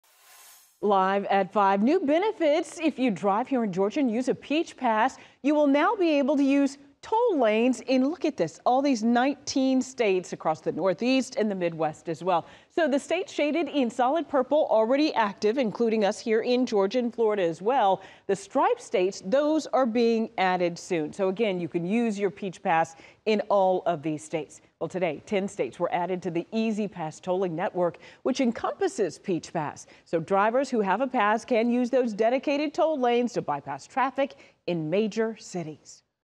Peach Pass now accepted in 10 more states
Drivers can now use Peach Pass in 10 more states, it was announced Wednesday.